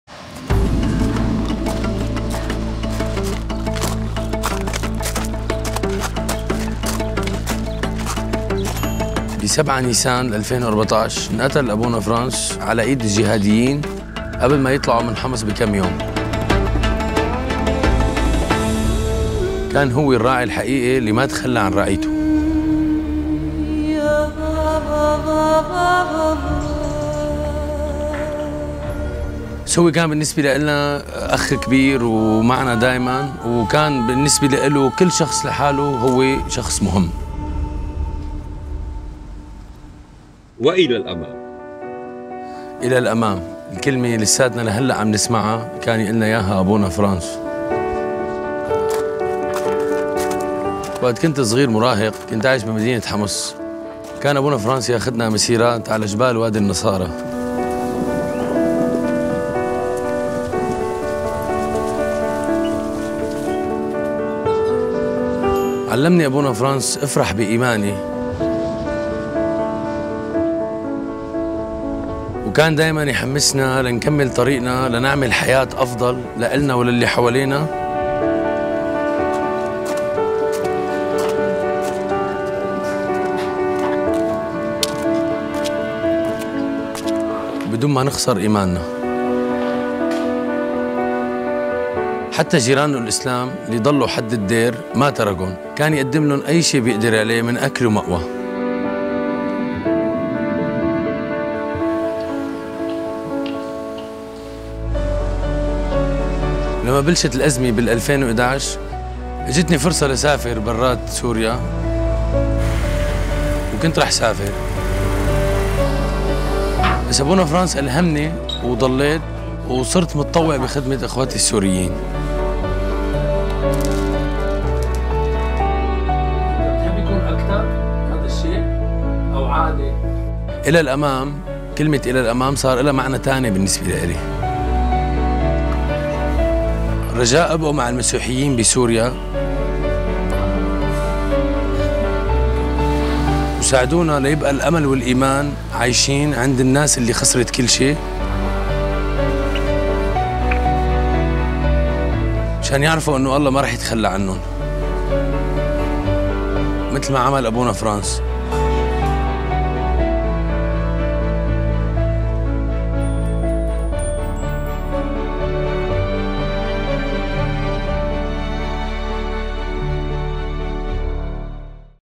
Padre Frans van der Lugt | Síria
O Padre Frans foi morto a tiro na sua paróquia em Homs por um jihadista na véspera de terem abandonado a cidade. Ele era um verdadeiro pastor que não queria abandonar o seu rebanho. Ele não se parecia connosco e, contudo, era nosso irmão, e olhava para cada um de nós como uma pessoa única. Para ele, cada pessoa interessava. Segue em frente! “Ila l-amam. Segue em frente.” Ainda estou a ouvir o Padre Frans a dizer estas palavras.
Quando eu era adolescente e vivia em Homs, ele levava-nos para longos passeios pelas montanhas.  Aprendi que há alegria na fé. Ele queria sempre que nós seguíssemos em frente, que mudássemos a nossa vida e a vida do nosso próximo para melhor. Ele não queria que perdêssemos a fé.
Também não abandonou os seus vizinhos muçulmanos que ficaram na cidade sitiada, oferecendo-lhes o  seu tecto e a pouca comida que tinha.

Quando o conflito começou em 2011, tive a oportunidade de emigrar para outro país. E quase que o fiz. Inspirado pela vida do Padre Frans fiquei e tornei-me voluntário para ajudar os meus irmãos e irmãs sírios. As suas palavras Ila l-amam, segue em frente, adquiriram um novo significado para mim.

Por favor, fique connosco, os Cristãos da Síria.
Ajude-nos a manter viva a esperança e a fé daqueles que perderam tudo;
para que eles saibam que Deus nunca os irá abandonar.

Como fazia o Padre Franz.

Lembre-se dos nossos irmãos e irmãs perseguidos!
Eles dão a sua vida pela nossa fé, todos os dias.